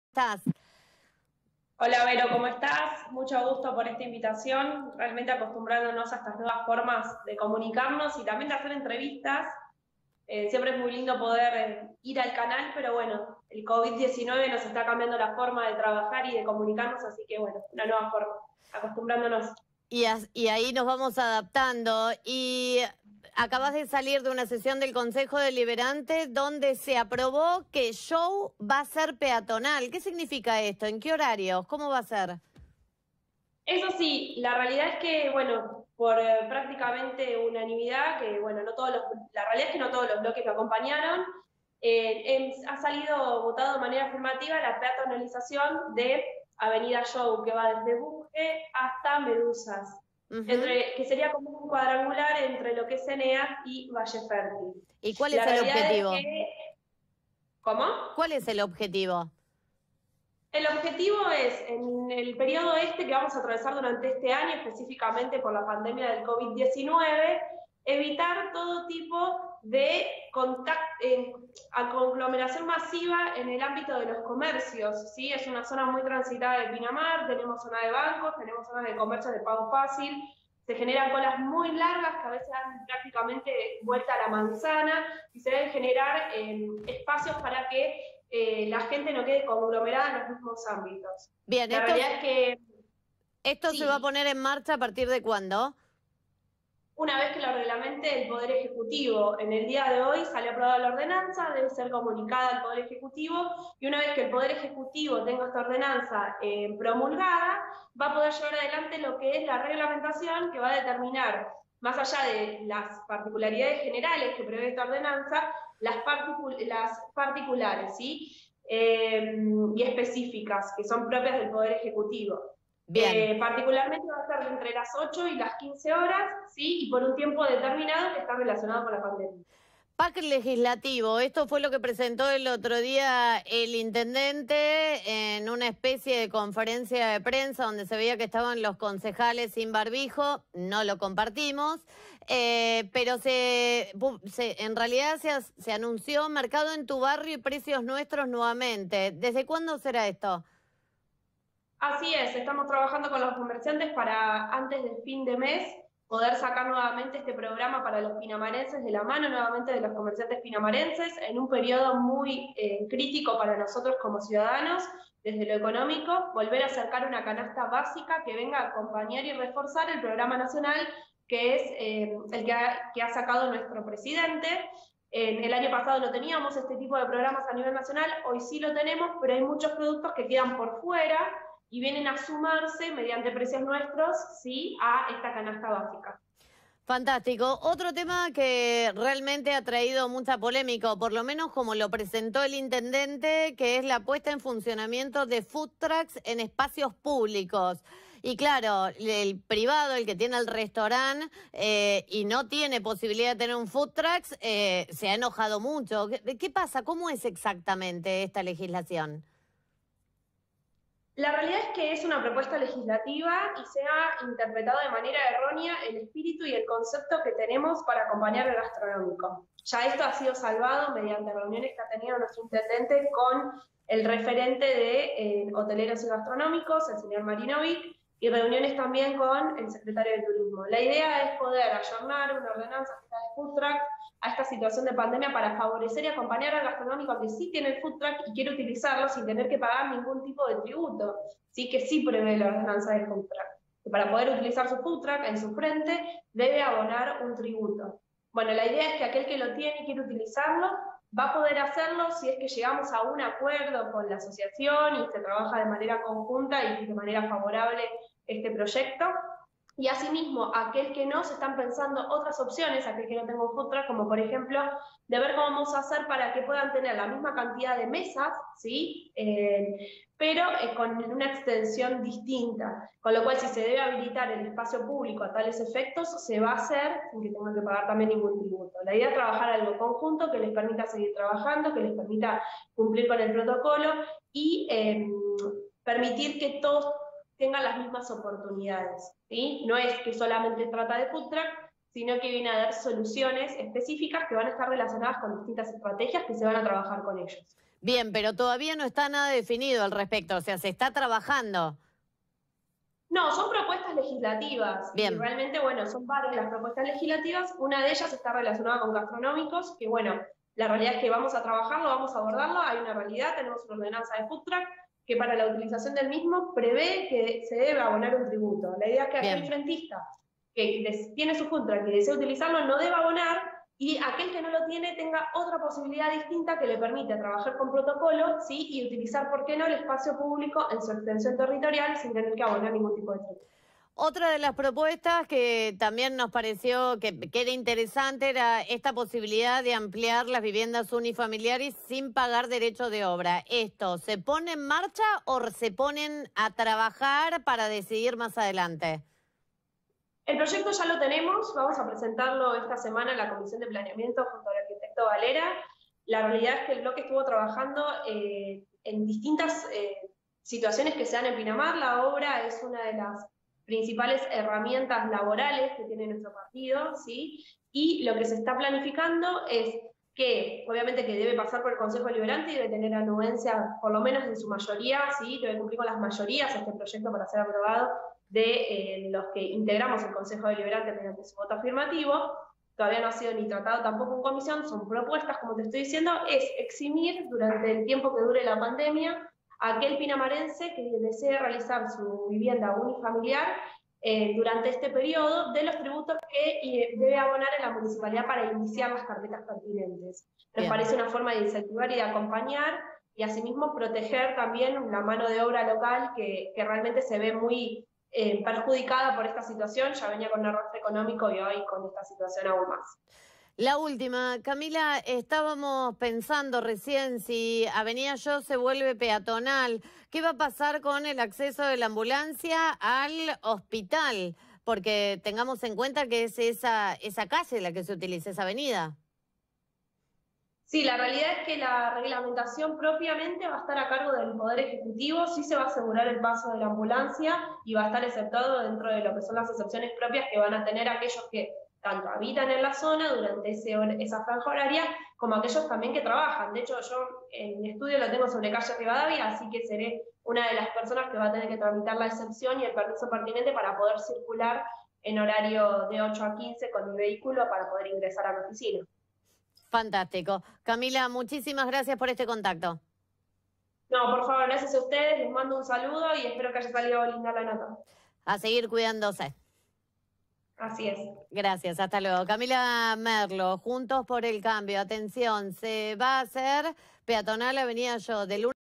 LA AVENIDA SHAW SERÁ PEATONAL
Camila Merlo, concejal de Juntos por el Cambio, aseguró que por decisión del HCD la Avenida Shaw será peatonal desde Bunge hasta Medusas (de 8 a 15hs), el tiempo que dure la pandemia. "El objetivo es evitar todo tipo de aglomeración masiva en el ámbito de los comercios, se forman filas muy largas y queremos generar espacios", dijo.  El ejecutivo pondrá en marcha esta ordenanza cuando reglamente los aspectos particulares y específicos.